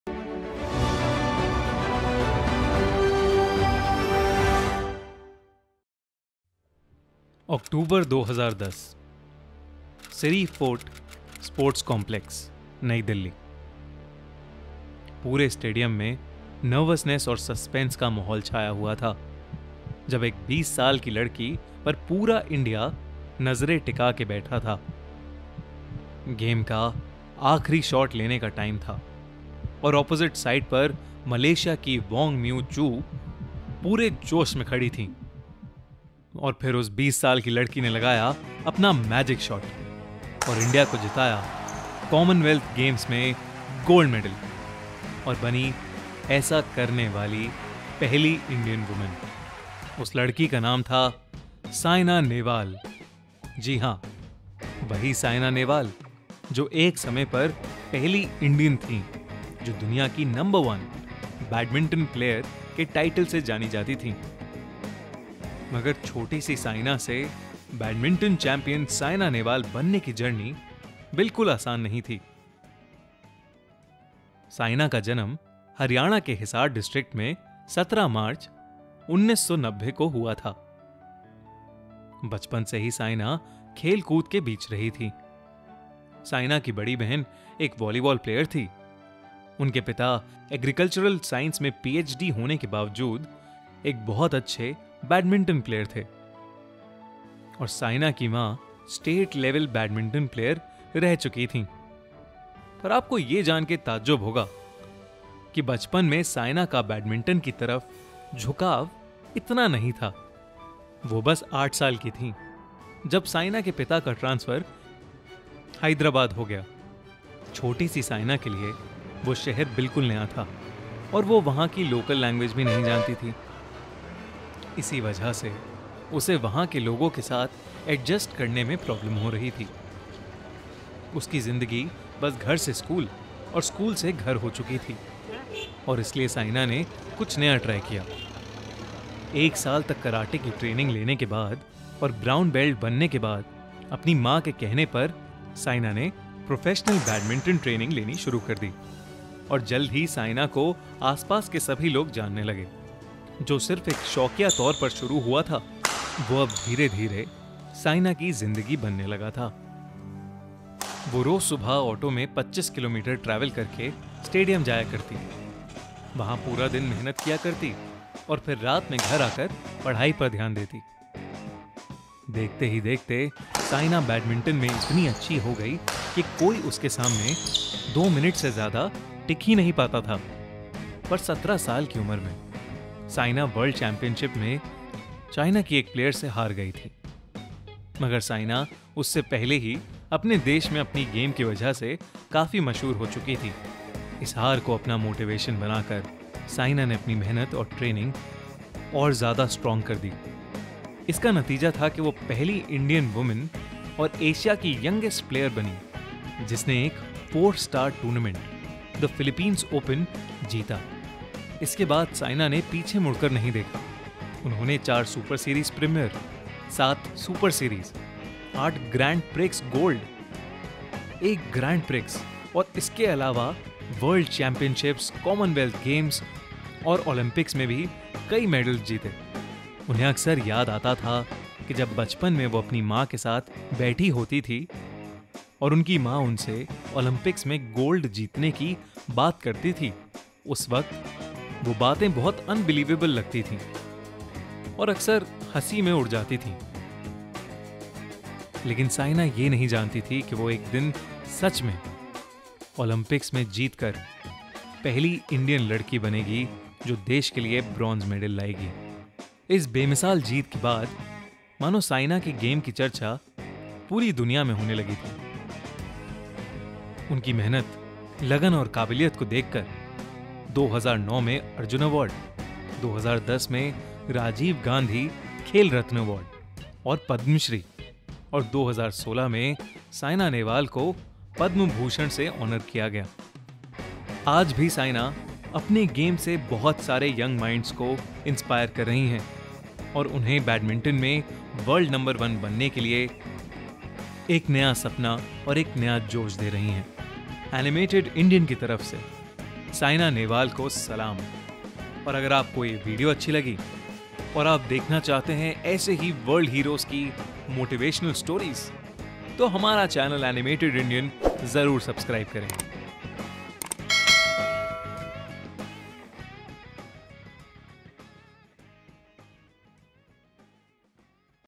Saina Nehwal Ki Real Inspirational Story | साइना नेहवाल | Hindi Kahaniyan | Motivational Videos
Saina Nehwal ki sachi kahani, Animated Indian ki zubani! Enjoy